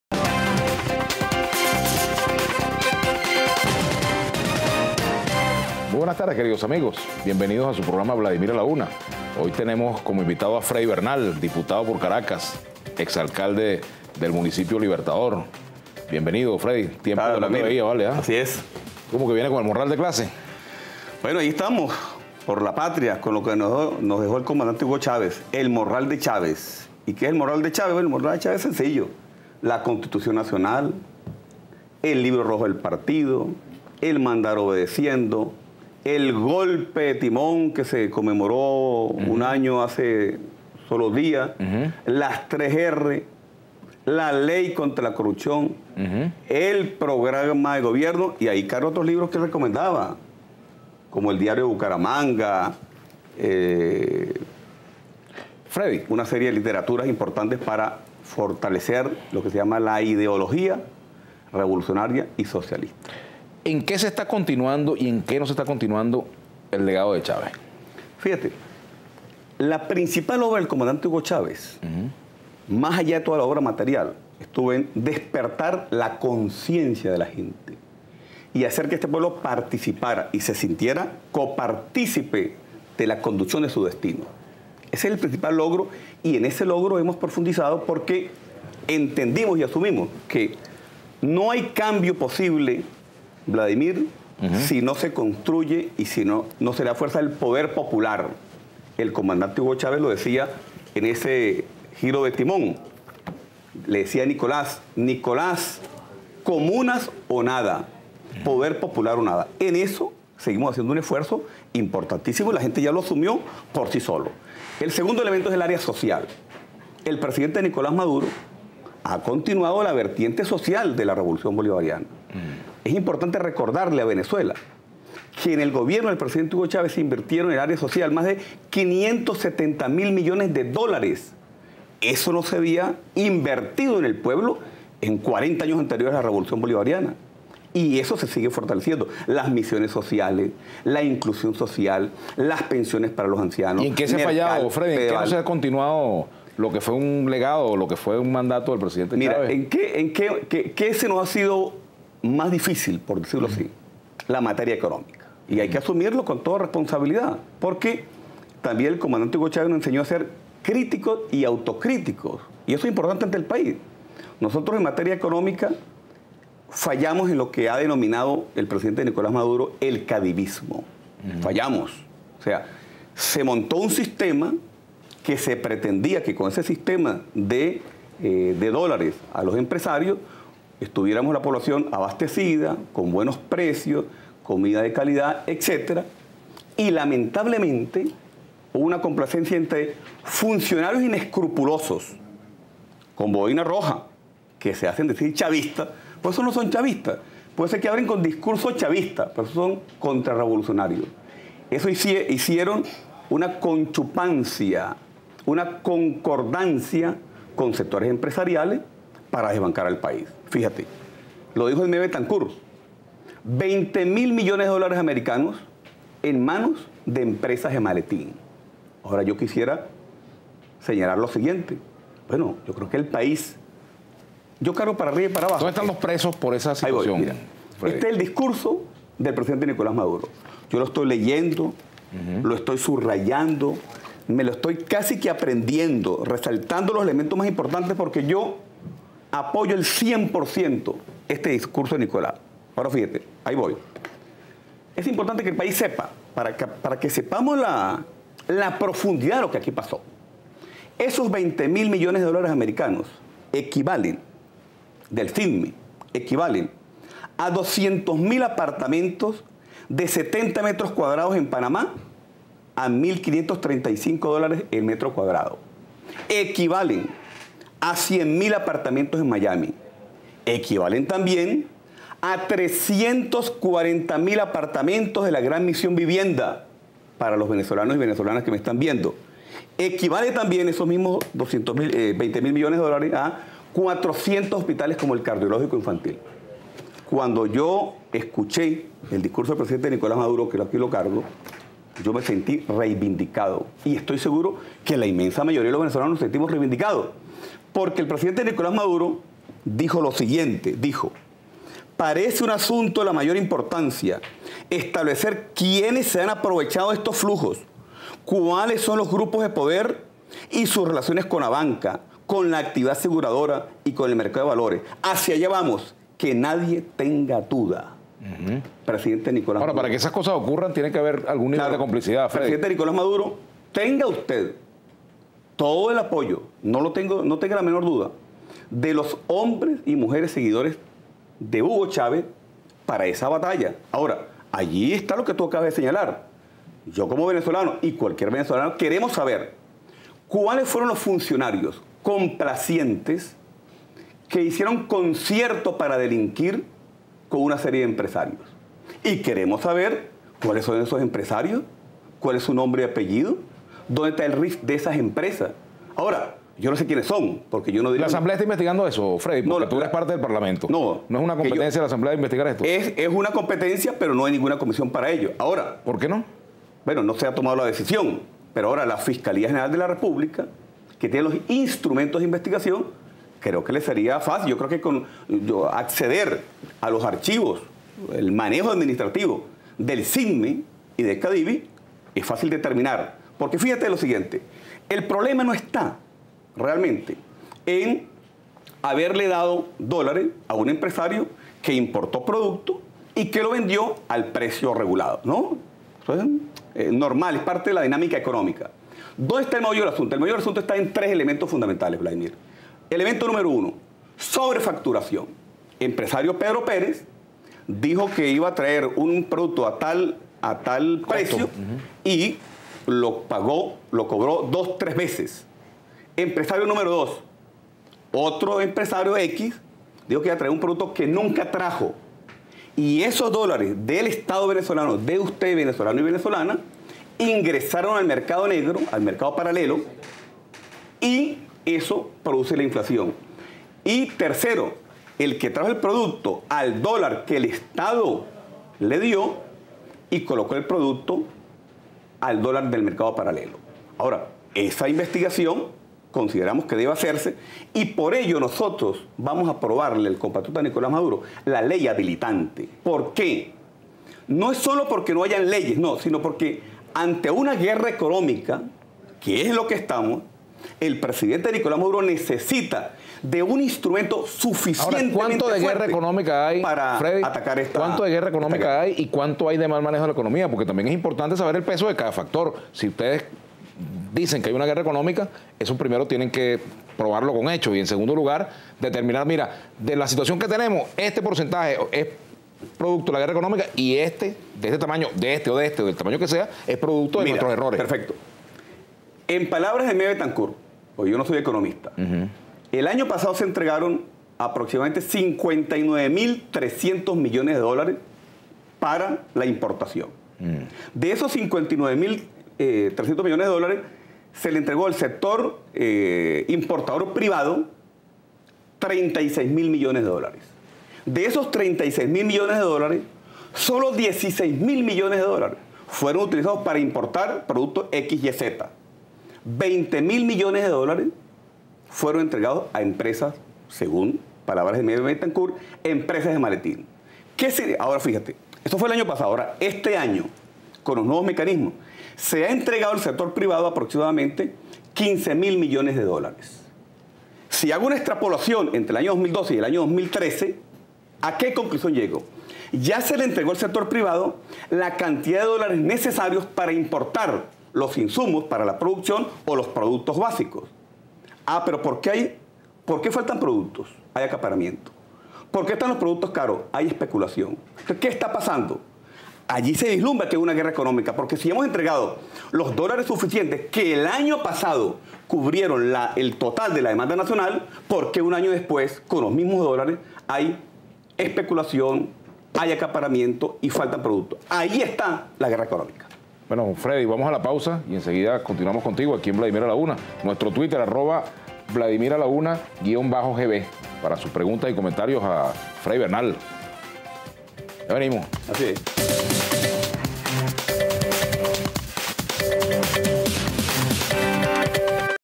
Freddy Bernal: "En el área económica hemos fallado con el cadivismo"
El diputado Freddy Bernal afirmó que el principal problema actual en la continuación de la gestión presidencial de Hugo Chávez radica en el área económica